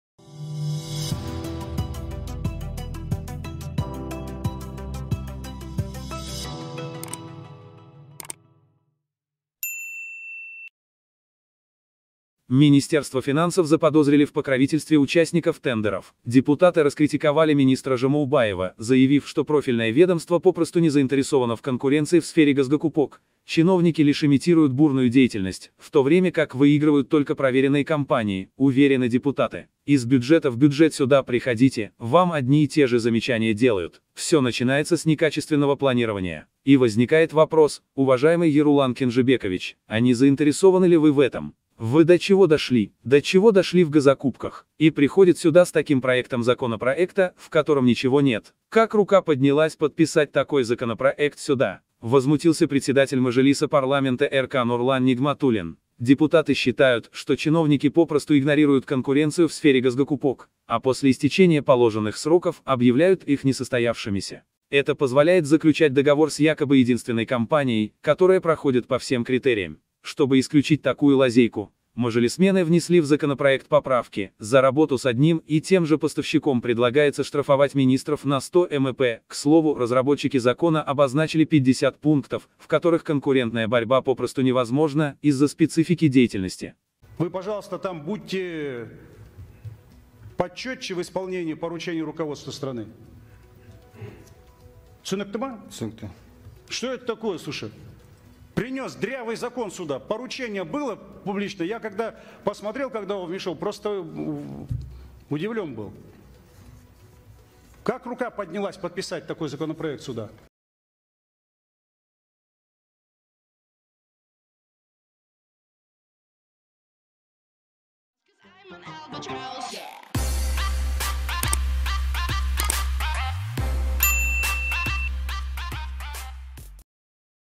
Нигматулин Рвёт и Мечет Как рука поднялась подписать такой законопроект
Нығматулиннің көз жасы және мешіт Мұндай заң жобасына қол қою үшін қол қалай көтерілді
Қаржы министрлігі сауда-саттыққа қатысушыларға демеушілік жасады деп күдіктенді. Депутаттар тиісті бөлімнің мемлекеттік сатып алу саласындағы бәсекелестік жай ғана мүдделі емес екенін айтып, министр Жамаубаевты сынға алды. Шенеуніктер тек зорлық-зомбылыққа еліктейді, дәлелденген компаниялар ғана жеңеді, ал депутаттар сенімді.
«Мұнда бюджеттен бюджетке келіңіз, олар сізге бірдей ескертулер жасайды. Барлығы жоспарлаудан басталады. Сұрақ туындайды, құрметті Ерұлан Кенжебекұлы: сізге бұл қызық па? Сіз қайда жеттіңіз? Мемлекеттік сатып алуларда қайда бардыңыз? Ол мұнда ештеңе жоқ заң жобасымен келеді. Мұнда осындай заң жобасына қол қою үшін қол қалай көтерілді? « - ҚР Парламенті Мәжілісінің төрағасы Нұрлан Нығматулин ашуланды. Депутаттар шенеуніктер мемлекеттік сатып алу саласындағы бәсекелестікті жай ғана елемейді және мерзім аяқталғаннан кейін оларды жарамсыз деп санайды деп санайды.
Бұл барлық өлшемдерге сәйкес келетін жалғыз компаниямен келісімшарт жасауға мүмкіндік береді. Мұндай олқылықты болдырмау үшін мәжілісмендер заң жобасына түзетулер енгізді: министрлерге бір жеткізушімен жұмыс істегені үшін 100 АЕК айыппұл салу ұсынылды. Айтпақшы, заң жасаушылар бәсекелестіктің жай мүмкін емес болатын 50 тармағын анықтады - бұл қызметтің ерекшеліктеріне байланысты.

Министерство финансов заподозрили в покровительстве участников тендеров. Депутаты раскритиковали министра Жамаубаева, заявив, что профильное ведомство попросту не заинтересовано в конкуренции в сфере госзакупок. Чиновники лишь имитируют бурную деятельность, в то время как выигрывают только проверенные компании, уверены депутаты.
«Из бюджета в бюджет сюда приходите, вам одни и те же замечания делают. Все начинается с некачественного планирования. И возникает вопрос, уважаемый Ерулан Кенжебекович: а не заинтересованы ли вы в этом?
Вы до чего дошли? До чего дошли в госзакупках? И приходит сюда с таким проектом законопроекта, в котором ничего нет. Как рука поднялась подписать такой законопроект сюда?» — возмутился председатель мажилиса парламента РК Нурлан Нигматулин. Депутаты считают, что чиновники попросту игнорируют конкуренцию в сфере госзакупок, а после истечения положенных сроков объявляют их несостоявшимися.
Это позволяет заключать договор с якобы единственной компанией, которая проходит по всем критериям. Чтобы исключить такую лазейку, мажилисмены внесли в законопроект поправки: за работу с одним и тем же поставщиком предлагается штрафовать министров на 100 МРП.
К слову, разработчики закона обозначили 50 пунктов, в которых конкурентная борьба попросту невозможна — из-за специфики деятельности.

💣Новый способ заработка в интернете.💣